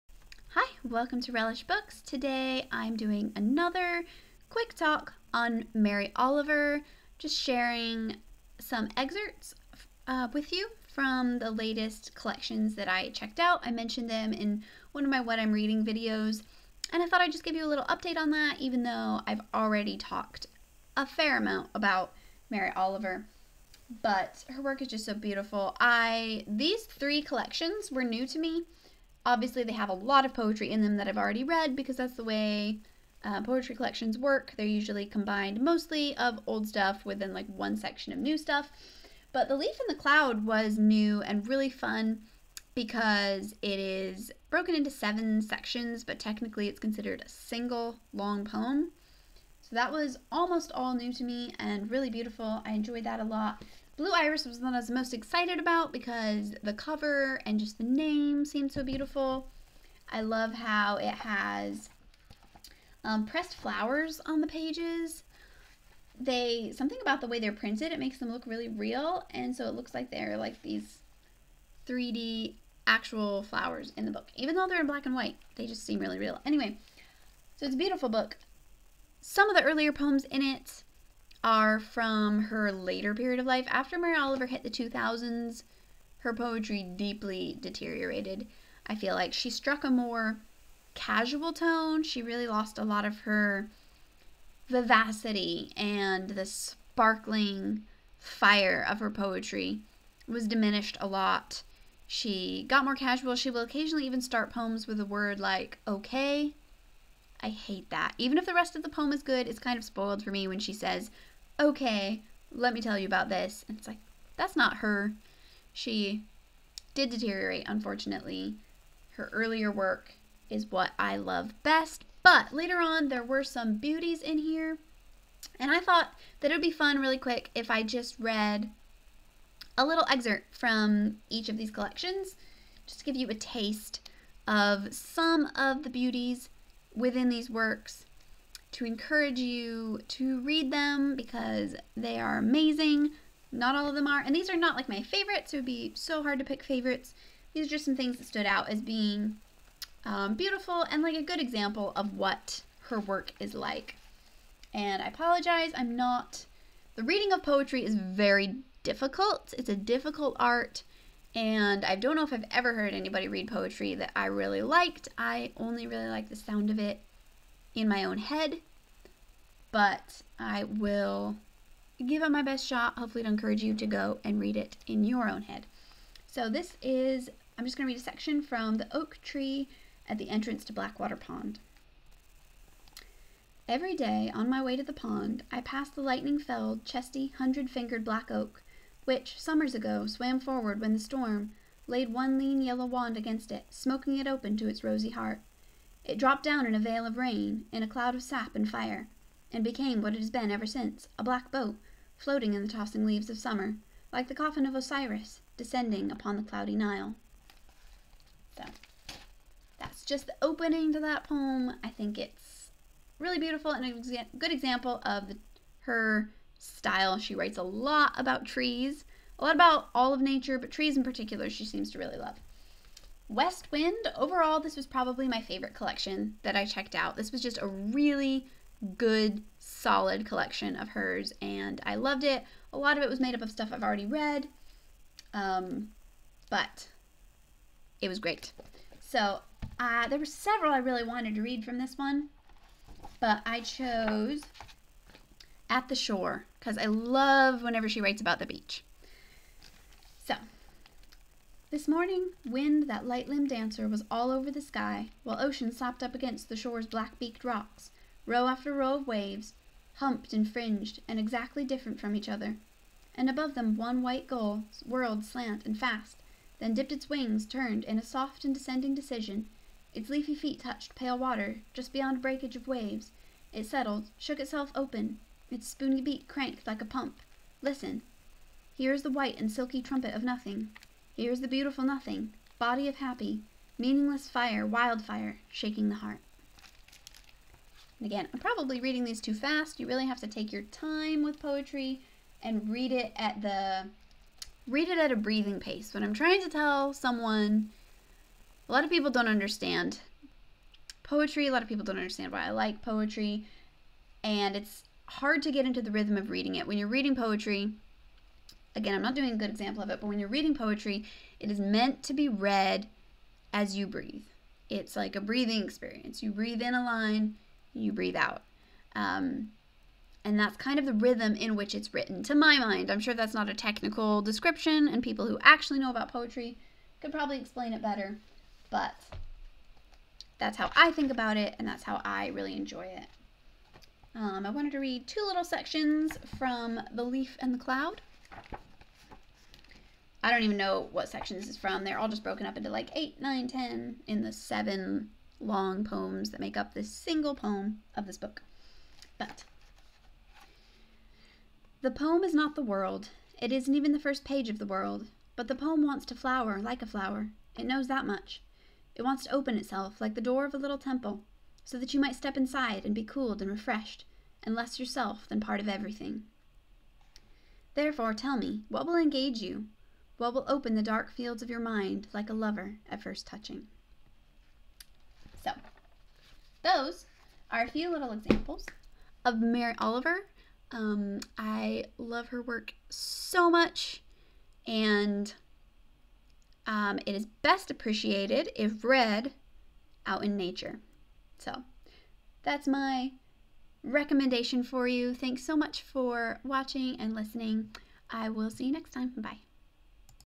More Mary Oliver!!
Sharing some excerpts from my latest Mary Oliver collections.